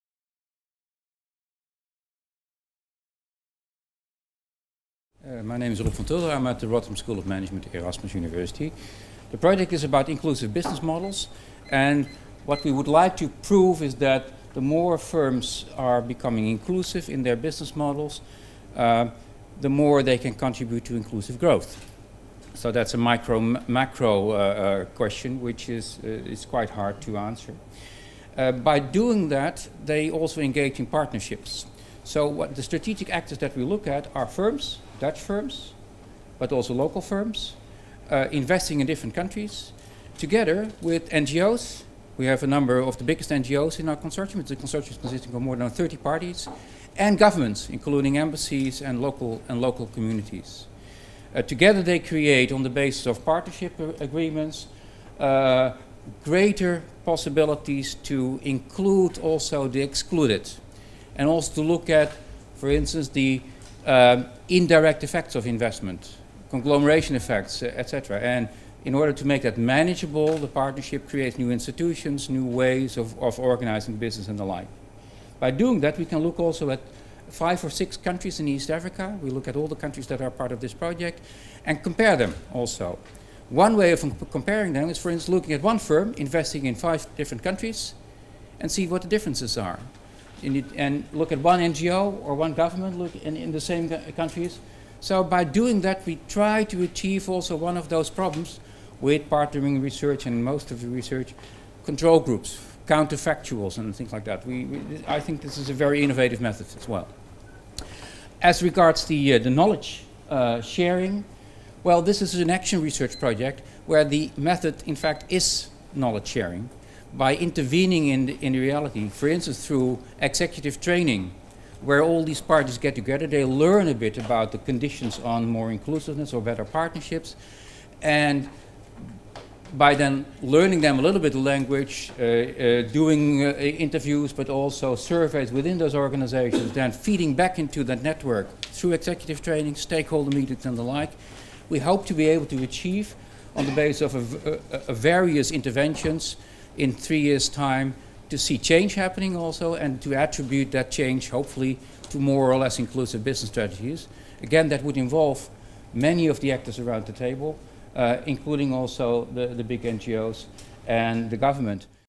Research Pitch Rob van Tulder
How Inclusive Business Strategies Can Contribute to Inclusive Development in Sub-Saharan Africa